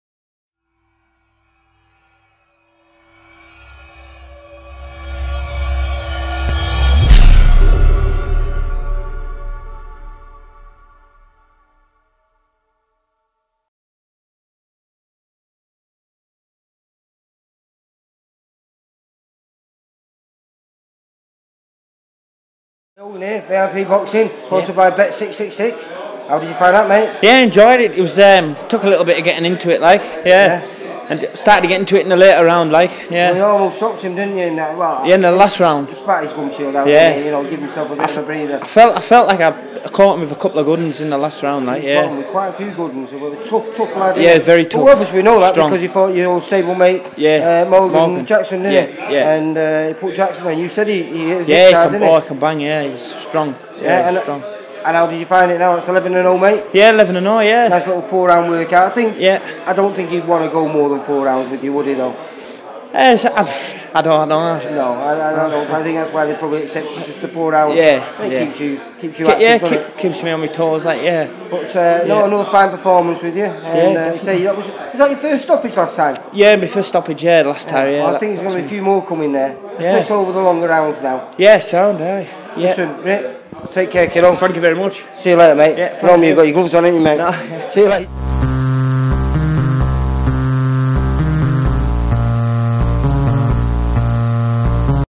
Ricky Skelton, Post Fight Interview - October 2014
Lee Hogan from VIP Boxing Promotions conducts a post fight interview with Ricky Skelton, following his fight on October 18th 2014